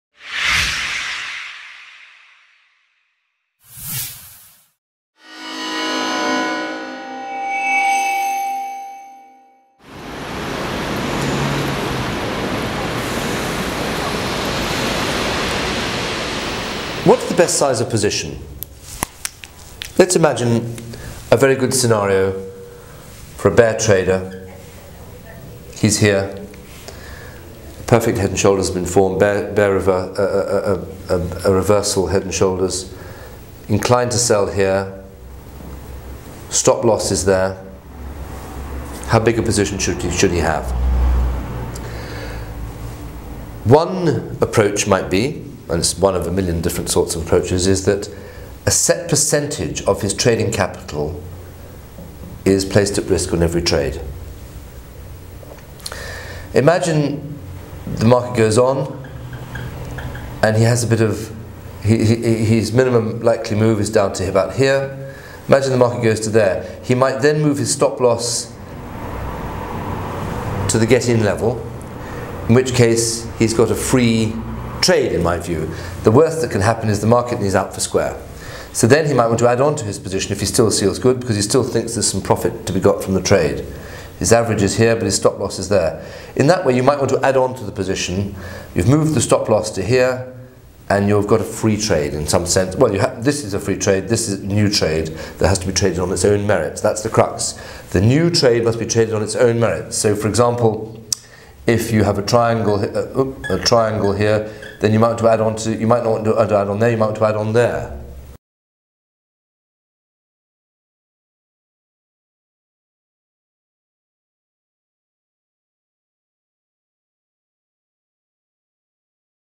Best Approach to Position Sizing?
What is the best size per position? One approach out of several money management systems is to risk a set percentage of capital on each trade.  Having locked in a profit, you can again risk a set percentage of your trading capital.